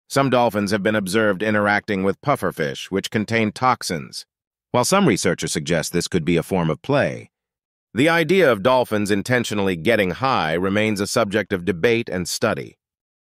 SHOCKING! Dolphins and Pufferfish: Playful Encounters or Seeking a High?
Delve into the intriguing behavior of dolphins as they interact with pufferfish, which are known to contain toxins. Explore the ongoing debate and research surrounding whether these interactions are playful encounters or something more complex, as we unravel the mysteries of the ocean's most intelligent creatures